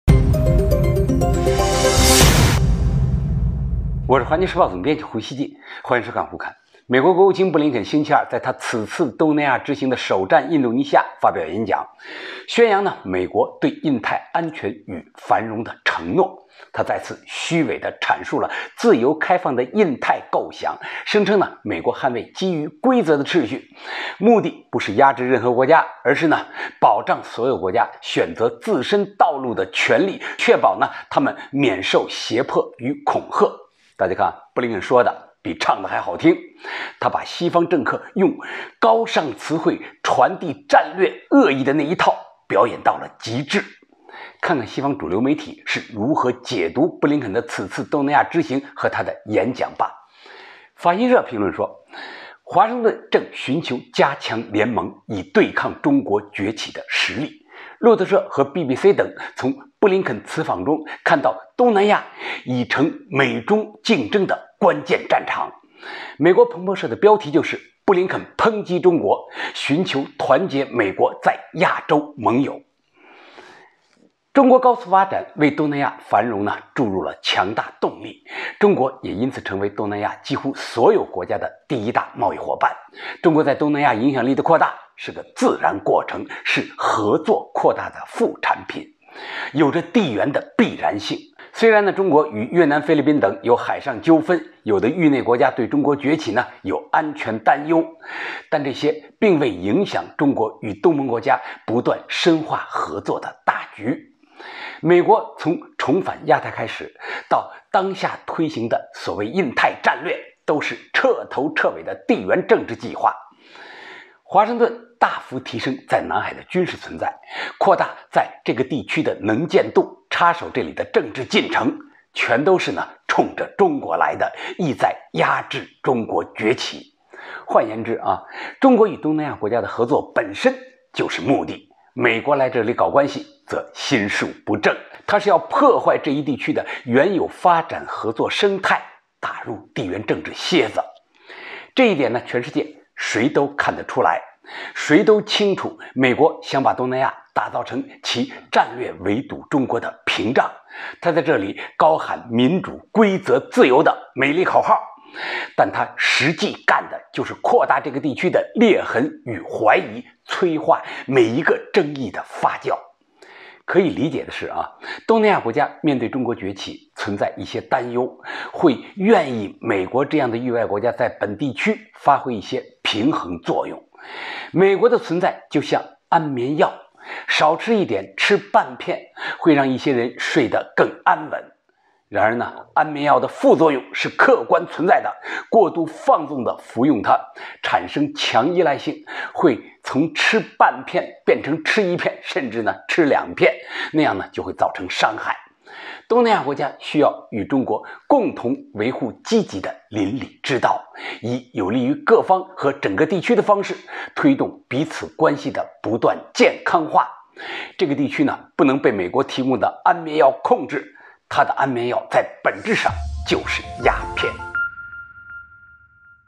东南亚，老美要生生把你变成“关键战场”【胡侃】
美国国务卿布林肯在印尼演讲，宣扬美国“对印太安全与繁荣的承诺”，目标直指中国。西媒评论称，东南亚已成美中竞争的“关键战场”。那么美国能把东南亚变成其对中国战略围堵的前沿屏障吗？